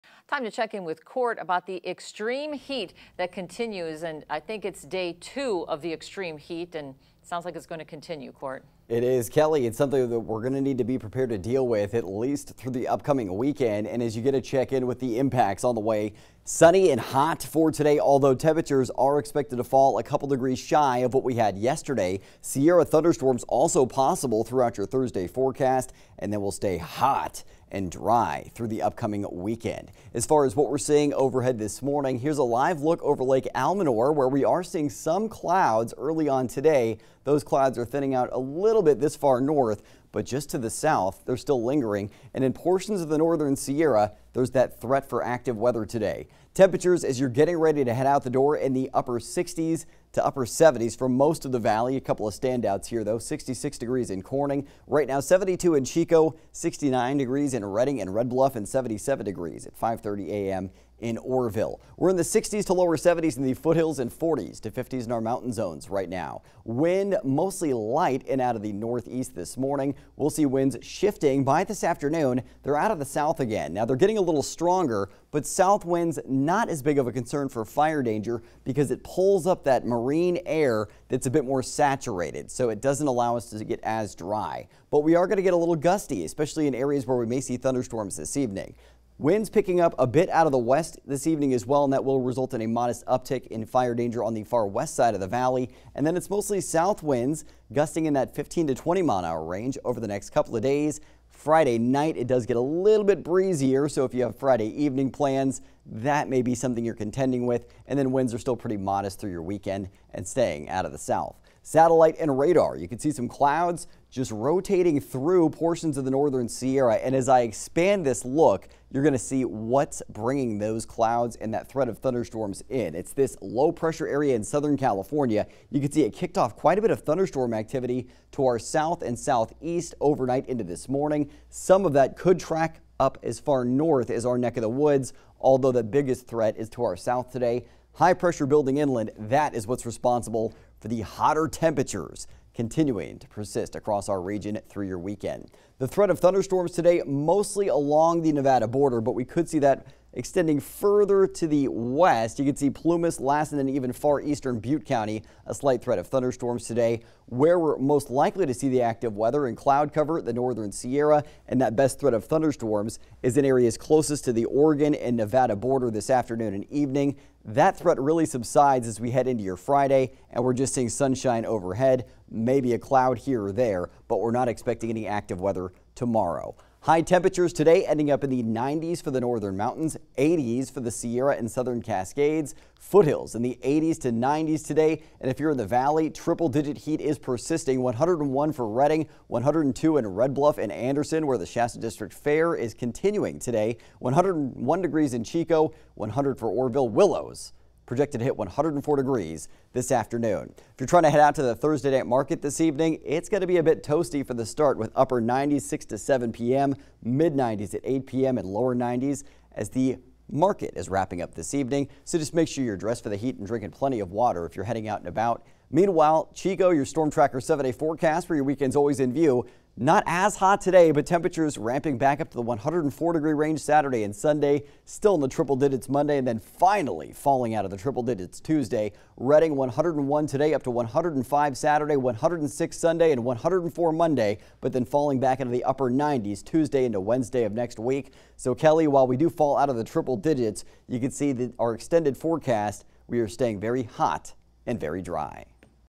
Storm Tracker Forecast: Toasty temperatures & the threat of Sierra Thunderstorms Thursday
Make sure to dress in lighter layers, grab your sunglasses and pack some extra water before you head out the door Thursday. Temperatures won't be as hot for your afternoon Thursday, but will still end up well above average. Most of our region will stay sunny, but the threat of thunderstorms will return to the Sierra. Sunny skies and very hot temperatures are projected to continue through your upcoming weekend.